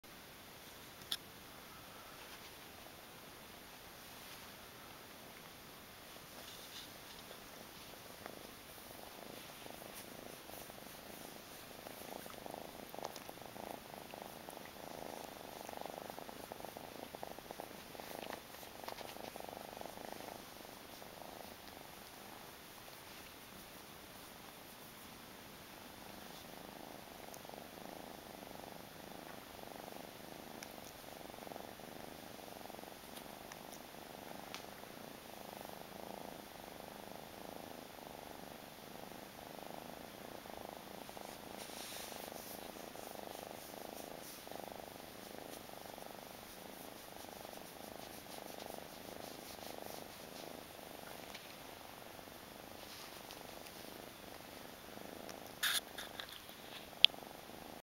Burmese kitten 1st day new home
Our new little girl having a cuddel on her first day with us